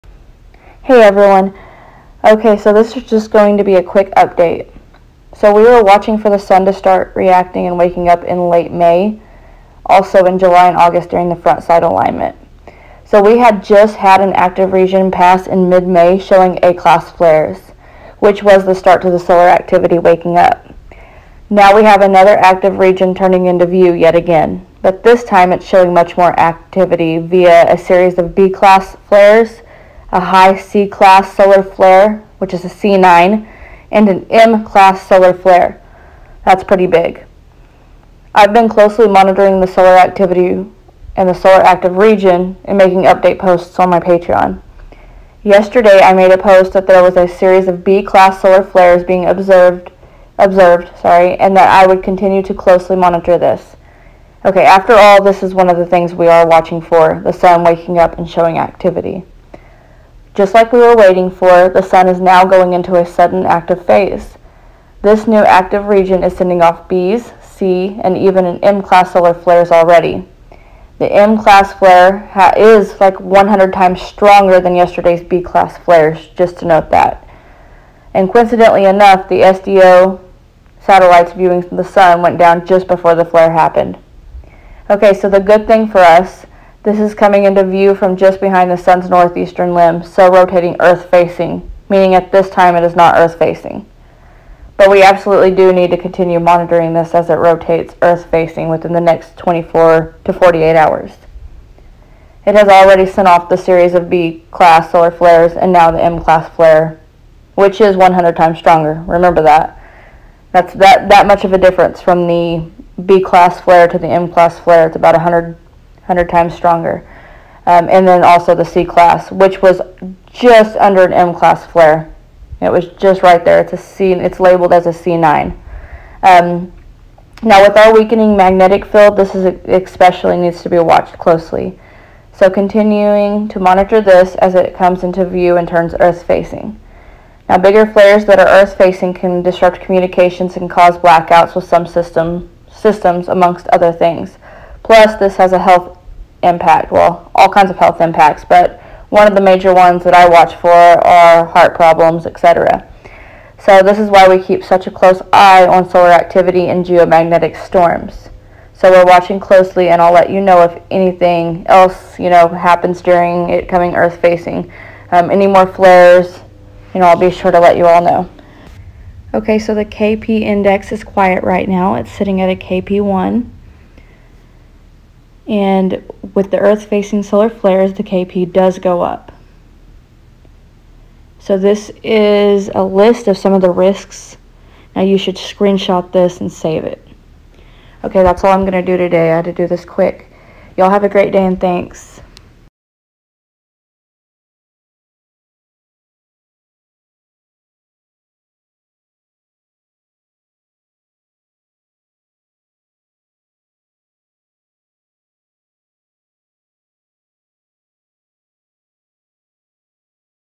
M Class Solar Flare / High C Class Flare (C9) / Series of B Class Flares - Continuing To Monitor
You can message me here if you have any questions:) thanks again 💕💫

(Sound messed up after upload )

UPDATE 4:07 central (my time)
Spaceweather Just Posted This
Great timing huh, given what we were watching for around this timeframe.

THE STRONGEST SOLAR FLARES SINCE 2017

For the first time in more than two years, the sun is really flaring. Today, May 29th, Earth-orbiting satellites detected an M1-class solar flare (0724 UT) followed by a C9-class flare (1046 UT).

Both came from a likely sunspot hiding just behind the sun's northeastern limb.

This image from NASA's Solar Dynamics Observatory shows material thrown up by one of the blasts

These are the strongest solar flares since Oct. 20, 2017 — The last time the sun produced an M-class eruption.

In fact, they might be even stronger than they seem. The explosions were partially eclipsed by the edge of the sun, reducing their apparent intensity.

So far the flares have not much affected Earth.
The underlying sunspot is facing away from our planet.

However, that could soon change.

Solar rotation will bring the 'spot over the limb within the next 24 to 48 hours.

Future flares could be geoeffective.

Reminder that SDO / NASA satellites went down moments before the flare at the wrong possible time leaving us almost blind

Watching for this active region to turn Earth Facing

- Laura Wells